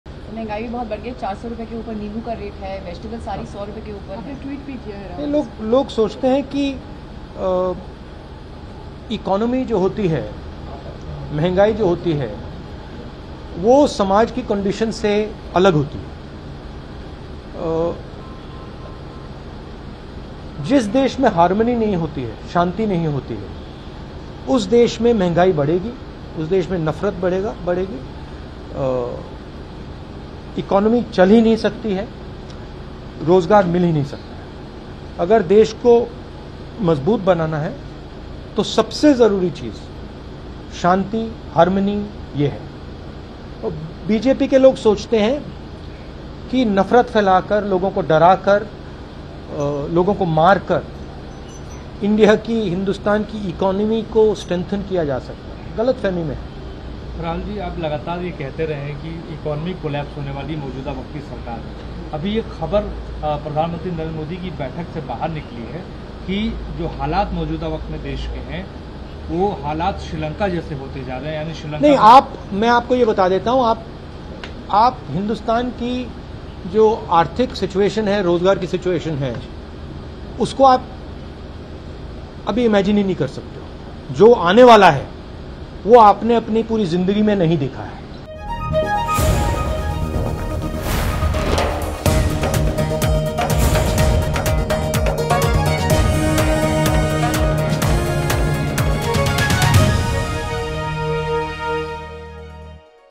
महंगाई पर Rahul Gandhi ने कहा- और बिगड़ेंगे हालात
राहुल गांधी (Rahul Gandhi) ने बढ़ती महंगाई (Inflation) पर मोदी सरकार पर हमला बोला है. उन्होंने कहा कि  आने वाला वक्त कैसा होगा इसका आप अंदाजा भी नहीं लगा सकते हैं.